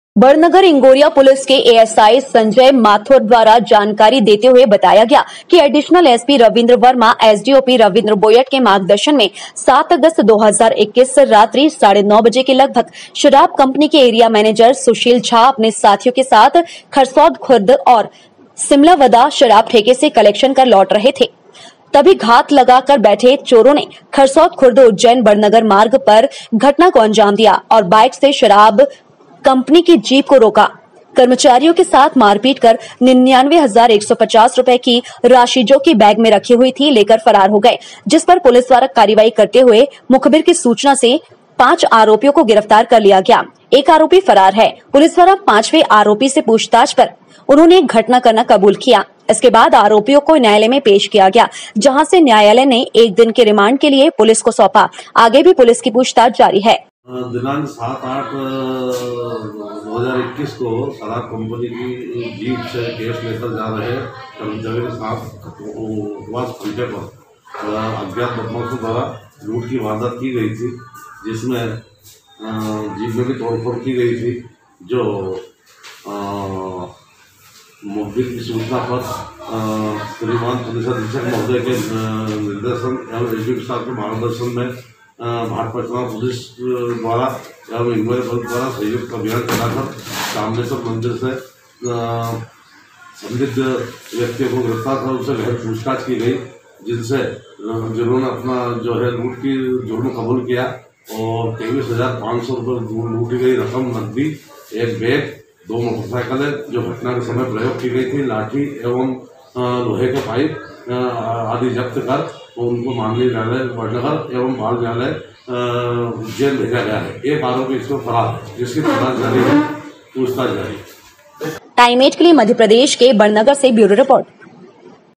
शराब कंपनी में हुई लूट का खुलासा।TIME8 MADHYA PRADESH
उज्जैन । इंगोरिया पुलिस को मिली सफलता शराब कंपनी के एरिया मैनेजर के साथ हुई लूट का किया खुलासा पांच आरोपियों को किया गिरफ्तार भाट पचलाना पुलिस का भी रहा सहयोग ₹23000 की राशि व दो बाइक की गई जप्त।